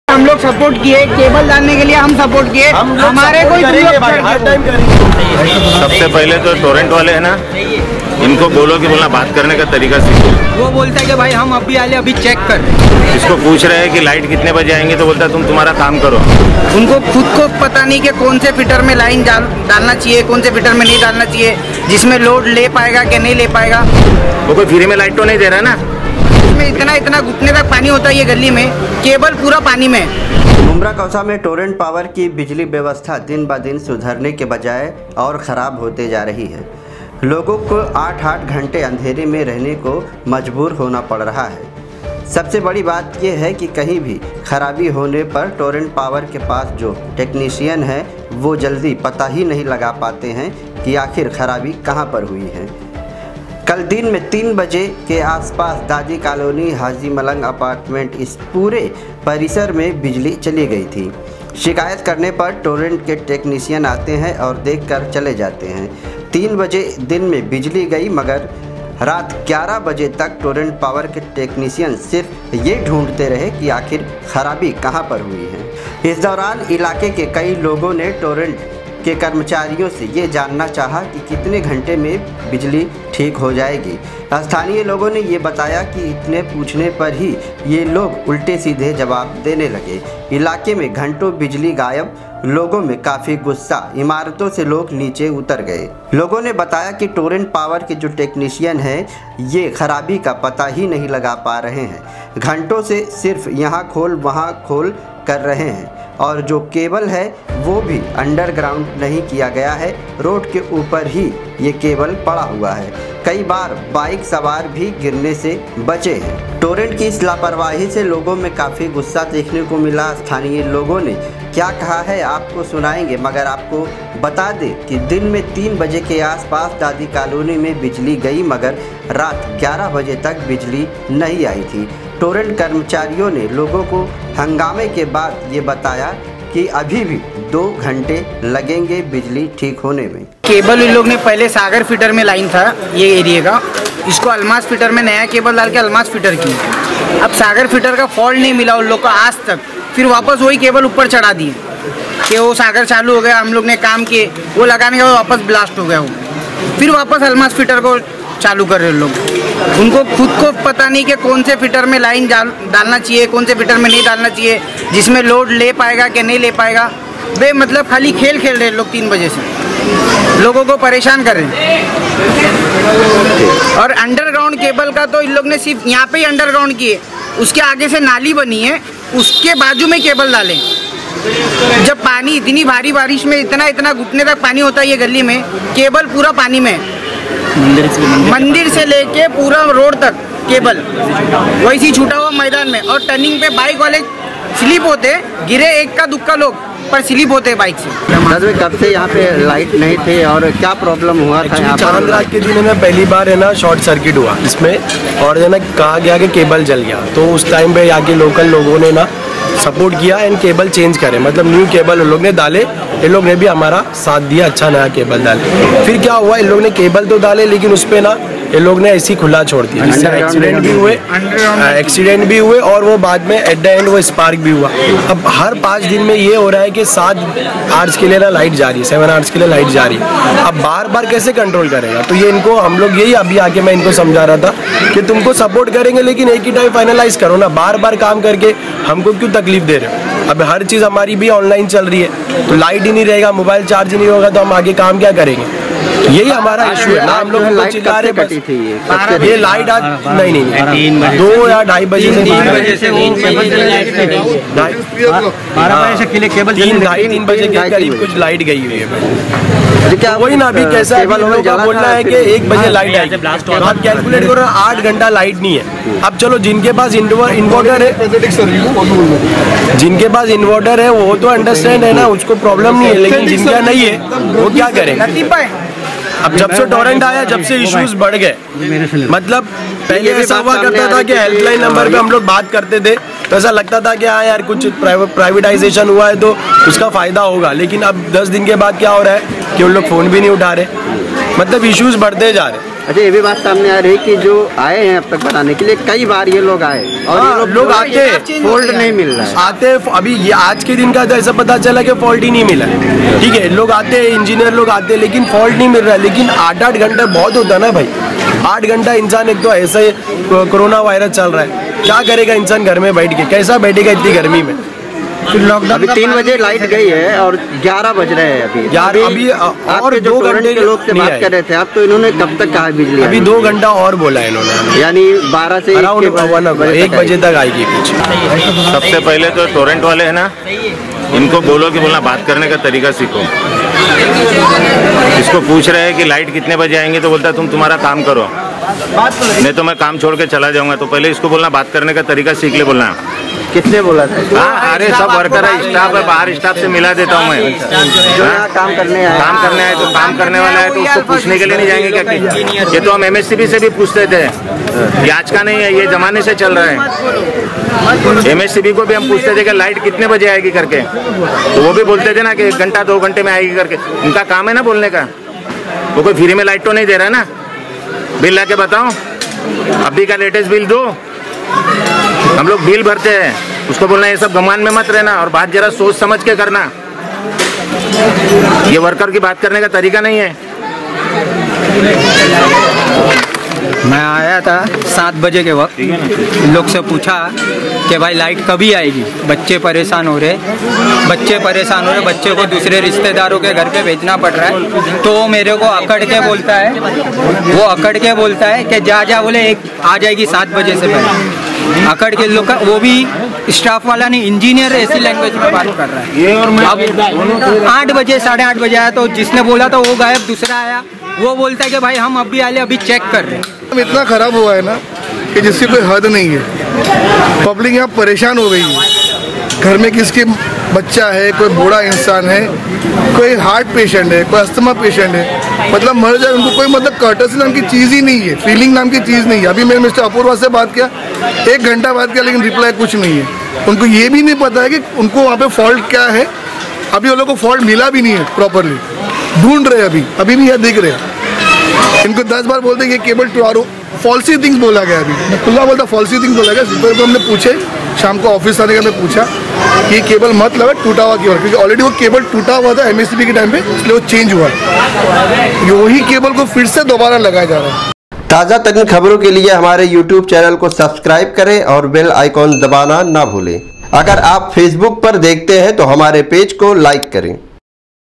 MUMBRA में बिजली कटने पर हंगामा टोरेंट हुई फेल | ISLAMUDDIN KHAN |Aapka Prahar Times
बिजली कटने पर हंगामा टोरेंट हुई फेल। ISLAMUDDIN KHAN
टोरेंट कर्मचारी लोगो से कर रहे गलत बर्ताव
मुम्ब्रा में टोरेंट पॉवर  पूरी  तरह से हुई फेल